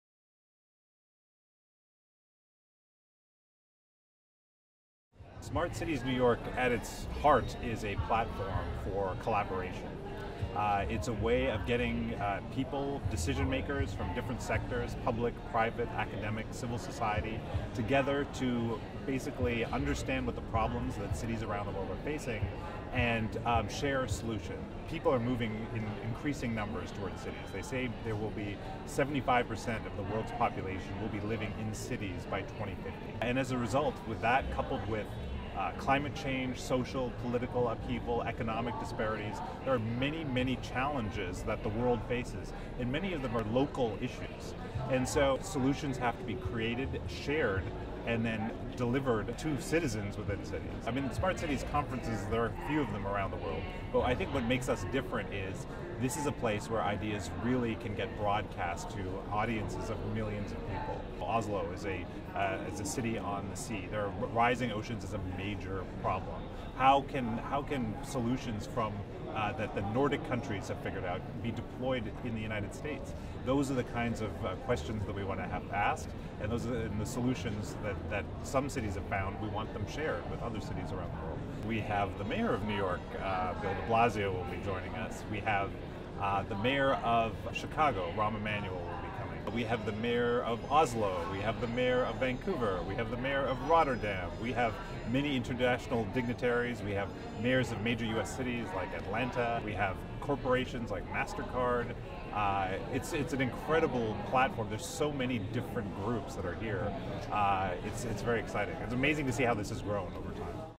Smart Cities New York 2018: Raj Pannu Interview
Interview with Raj Pannu, co-founder of Smart Cities New York